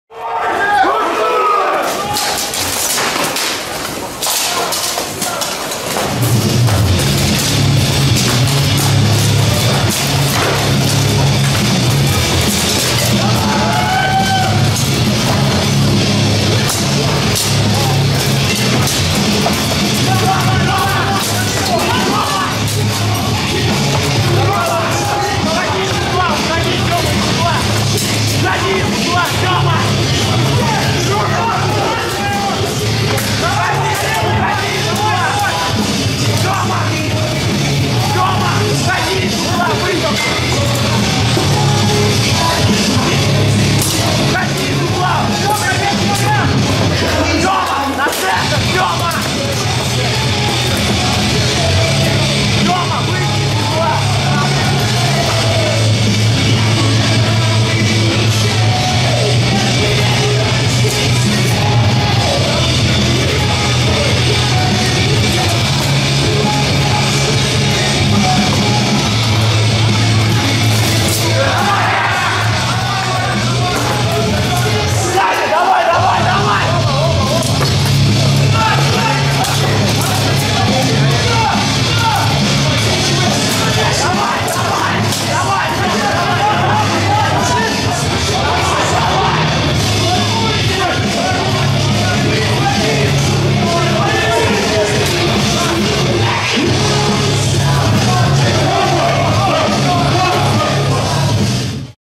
К Динамо 2012 2б Персик- З Башня 3-0 Moscow Dynamo Cup 2012 2f Юрий
Традиционный Кубок Динамо в Москве открывает серию отборочных 5х5 на Битву Наций 2013 .